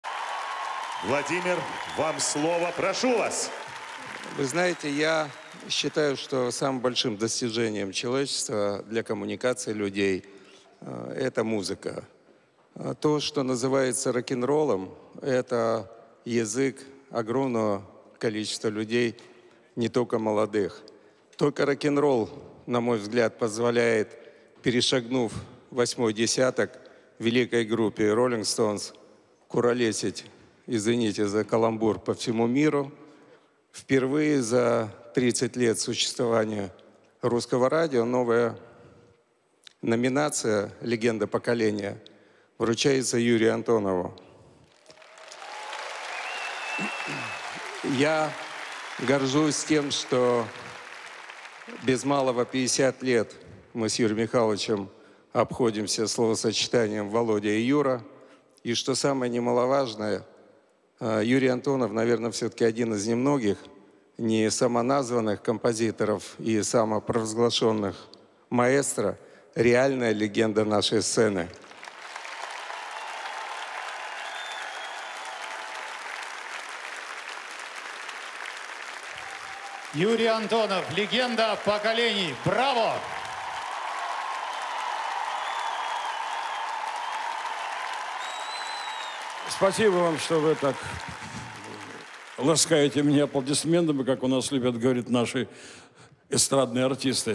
Награждение Юрия Антонова на национальной музыкальной премии "Золотой граммофон". 2021
Юрий Антонов стал обладателем специальной премии "Легенда поколений"